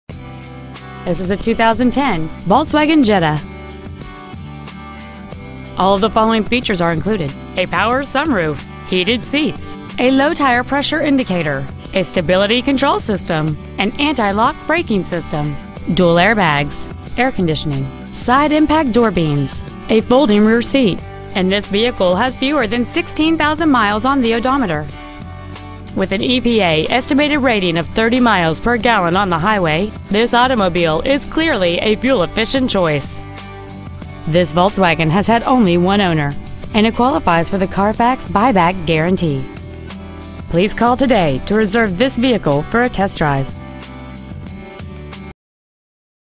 2010 Volkswagen Jetta Plainville CT 06062
Year : 2010
 Make : Volkswagen
 Model : Jetta
 Engine : 2.5L 5 Cyl. Sequential-Port F.I.
 Trans . : Not Specified
 Exterior : Blue
 Miles : 15,069

 Crowley Volkswagen
 Pre-Owned 2010 Volkswagen Jetta Plainville CT